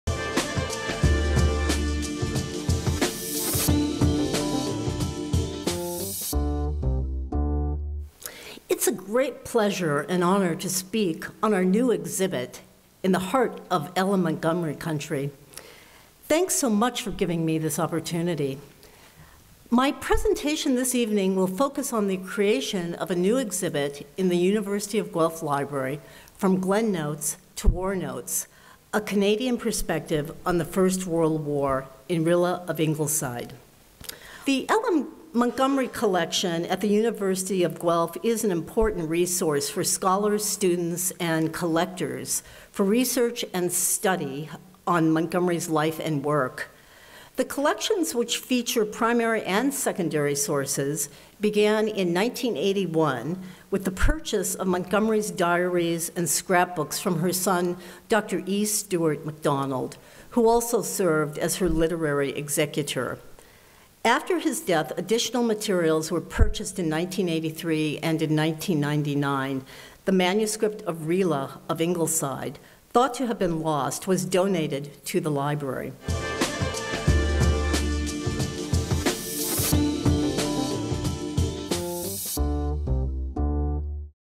HH Lecture Series - Lucy Maud Montgomery
The Halton Hills Lecture Series presents Melissa McAfee with her presentation on Lucy Maud Montgomery from the John Elliott Theatre in Georgetown, Ontario.